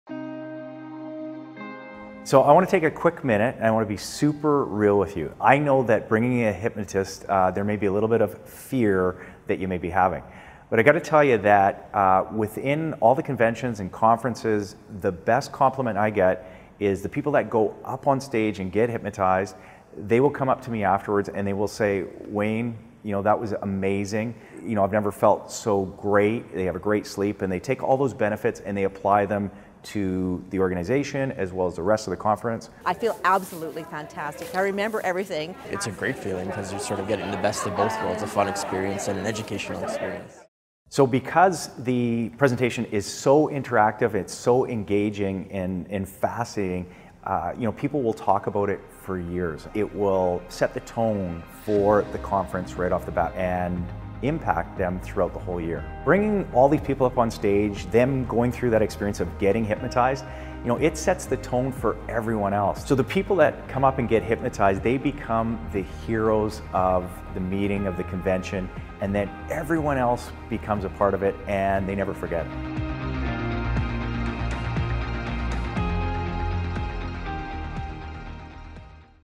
Hypnosis ... Scary or Fun?
In the world of corporate events bringing in a hypnotist can be concerning. I address the reasons why it will give wings to your meeting and make it unforgettable for your attendees.